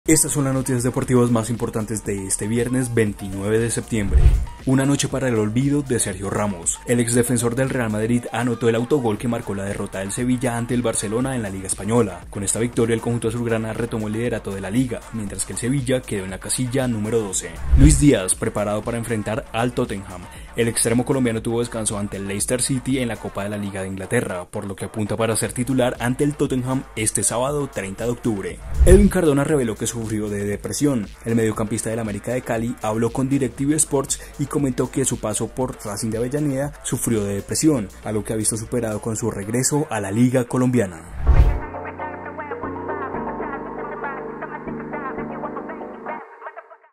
Edwin Cardona y la depresión que sufrió en Argentina
El mediocampista del América de Cali reveló que en su paso por Racing de Avellaneda sufrió de depresión, algo que ha visto superado con su regreso a la liga colombiana.